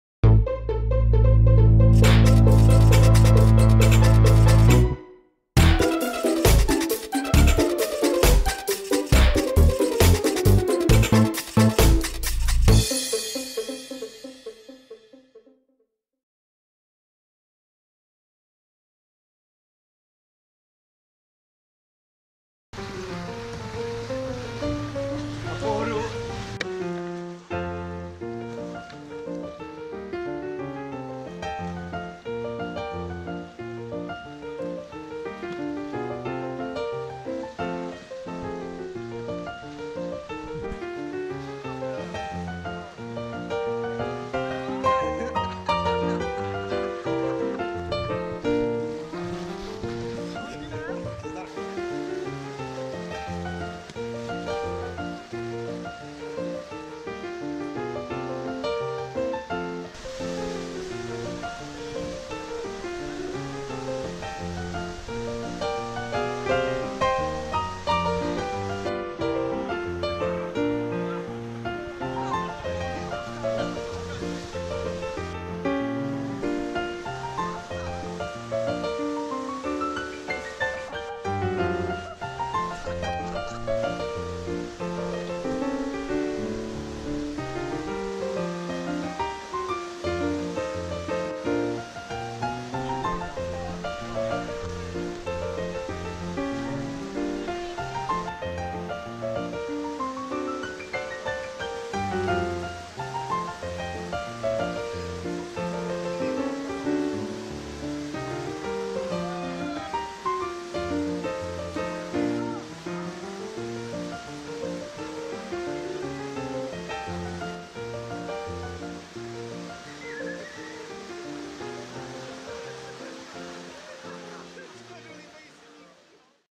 To nejlepší z Vavřince 2017
Krátká ukázka toho nejlepšího ze splutí potoka Vavřinec v roce 2017. Delší video se připravuje.